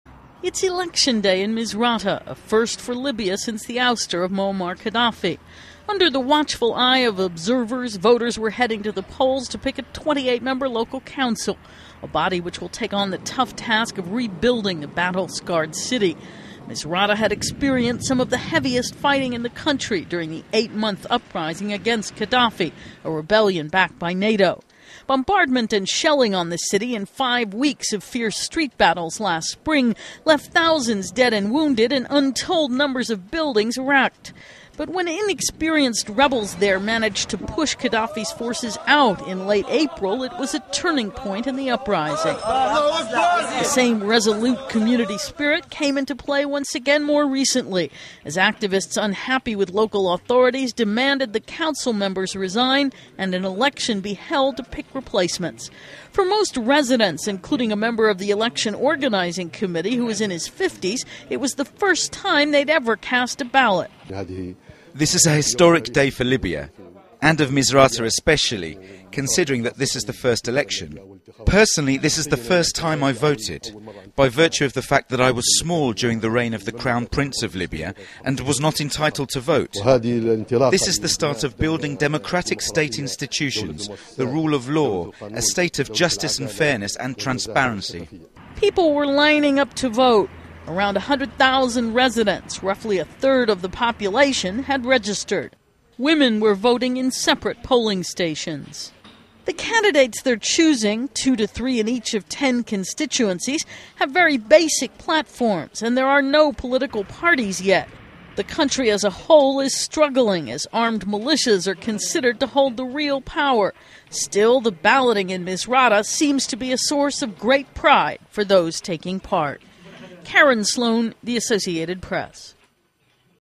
Libya: Voters Electing Council in Misrata
Residents of Misrata were at the polls Monday, casting ballots in a local council election. It was the first voting in Libya since the ouster of longtime leader Moammar Gadhafi. (Feb. 20)